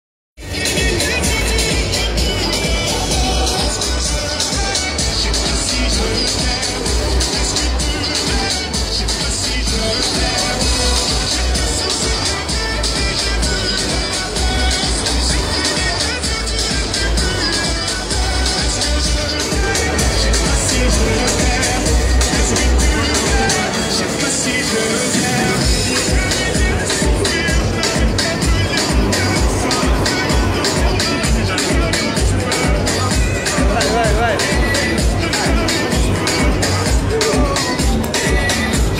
Ristopiù Lombardia RistorExpo Sfida di pizza acrobatica
Ristopiù Lombardia Spa torna al RistorExpo per l’edizione 2016 con tante novità e un’iniziativa imperdibile: Una pizza per la vita – Trofeo Ristopiù 2016 a favore de Il Ponte del Sorrsio Onlus.
La manifestazione è organizzata in collaborazione con l’Associazione Pizzaioli Varesini e con il Team Acrobatico Pizza Champs Italia che, insieme al Campione mondiale dei Pizzaioli Leone Coppola, ci terranno compagnia presso lo stand Ristopiù Lombardia con Dimostrazioni, Esibizioni, Forum, Degustazioni, Show Cooking e molto altro.